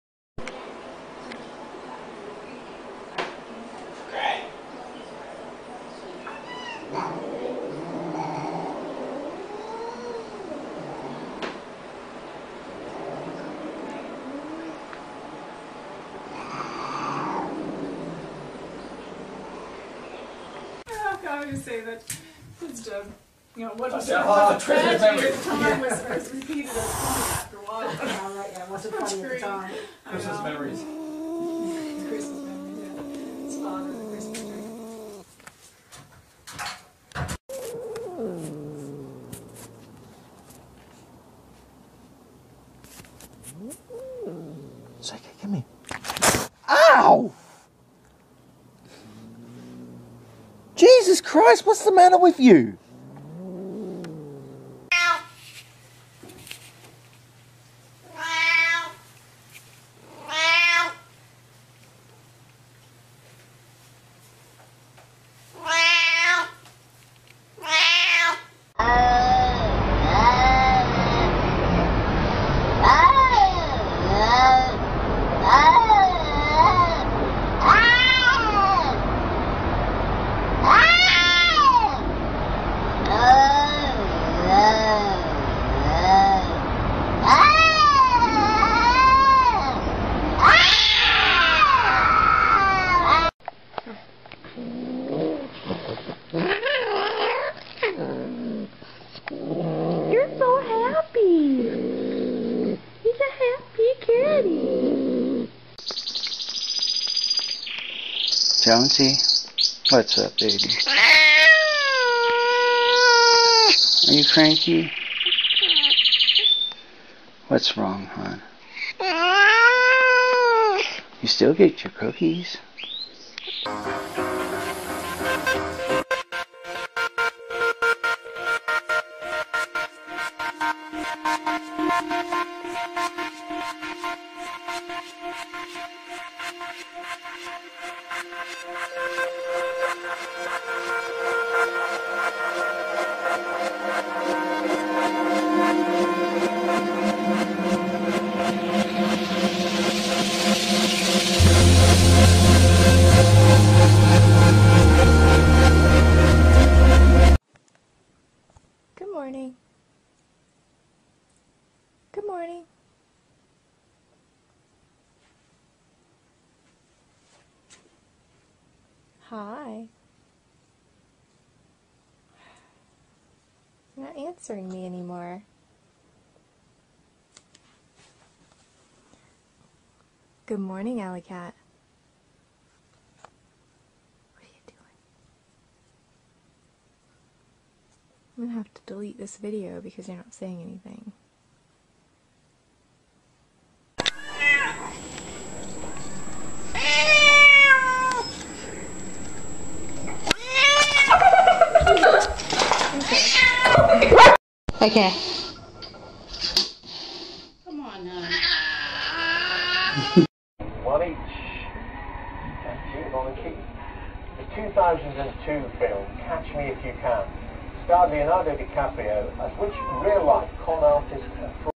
If You Get Scared You Lose - Angry And Grumpy Cats Compilation 2017
Angry and grumpy and mad cats it's not the best day for these cats at all :) Hope you liked this compilation. Thanks! Check out also our other channels and videos...